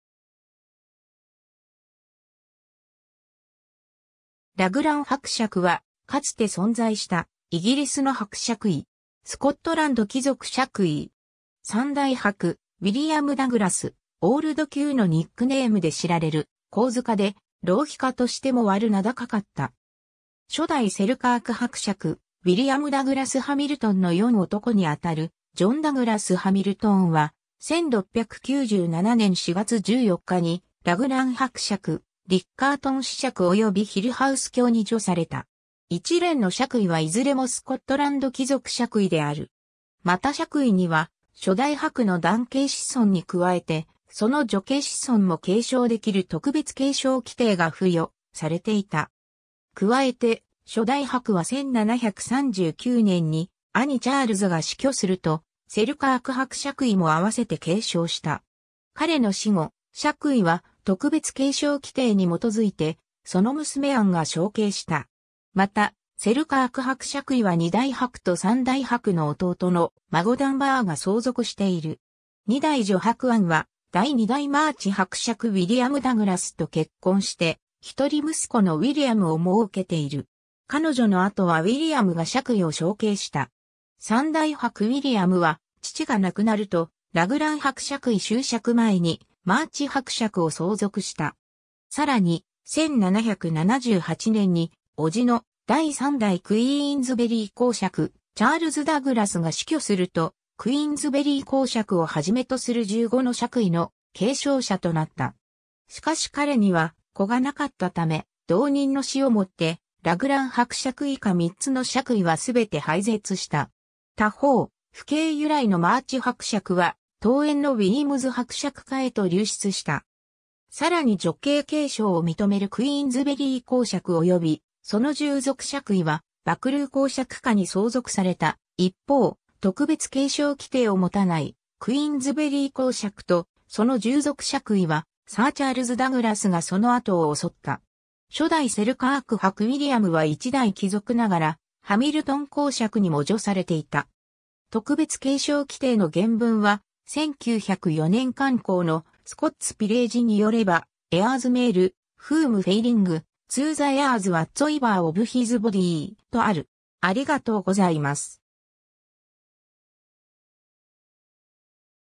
ラグラン伯爵
ラグラン伯爵(英:Earl of Ruglen)は、かつて存在したイギリスの伯爵位。
スコットランド貴族爵位。
 3代伯ウィリアム・ダグラス 『オールド・Q』のニックネームで知られる好事家で、浪費家としても悪名高かった。
 初代セルカーク伯爵ウィリアム・ダグラス・ハミルトン(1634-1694)の四男にあたるジョン・ダグラス＝ハミルトン(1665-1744)は、1697年4月14日にラグラン伯爵(Earl of Ruglen)、リッカートン子爵(Viscount of Riccartoun)及びヒルハウス卿(Lord Hillhouse)に叙された。
一連の爵位はいずれもスコットランド貴族爵位である。
また爵位には初代伯の男系子孫に加えて、その女系子孫も継承できる特別継承規定が付与されていた。
 加えて、初代伯は1739年に兄チャールズが死去すると、セルカーク伯爵位も併せて継承した。
 彼の死後、爵位は特別継承規定に基づいて、その娘アンが承継した。
またセルカーク伯爵位は2代伯と3代伯の弟の孫ダンバーが相続している。
 2代女伯アン(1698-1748)は第2代マーチ伯爵ウィリアム・ダグラスと結婚して、一人息子のウィリアムを儲けている。
彼女の後はウィリアムが爵位を承継した。
 3代伯ウィリアム(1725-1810)は父が亡くなると、ラグラン伯爵位襲爵前にマーチ伯爵を相続した。
さらに、1778年に叔父の第3代クイーンズベリー公爵チャールズ・ダグラスが死去すると、クイーンズベリー公爵を始めとする15の爵位の継承者となった。
 しかし彼には子がなかったため、同人の死を以てラグラン伯爵以下3つの爵位はすべて廃絶した。
他方、父系由来のマーチ伯爵は遠縁のウィームズ伯爵家へと流出した。
さらに女系継承を認めるクイーンズベリー公爵及びその従属爵位はバクルー公爵家に相続された一方、特別継承規定を持たないクイーンズベリー侯爵とその従属爵位はサー・チャールズ・ダグラスがその後を襲った。
 ^ 初代セルカーク伯ウィリアムは一代貴族ながらハミルトン公爵にも叙されていた。
 ^ 特別継承規定の原文は、1904年刊行の『スコッツ・ピレージ(The Scots Peerage)』によれば、『heirs male, whom failing, to the heirs whatsoever of his body.』とある。